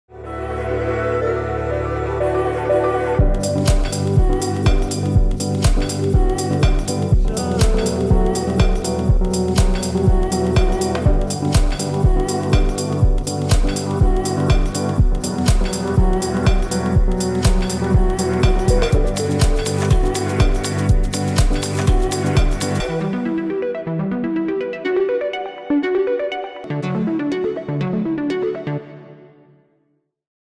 REMINGTON PROluxe Anwendungsvideo CI9132
150 Produkttester testen die REMINGTON® PROluxe Haartstyling-Serie für besonders lang anhaltende Styling-Ergebnisse!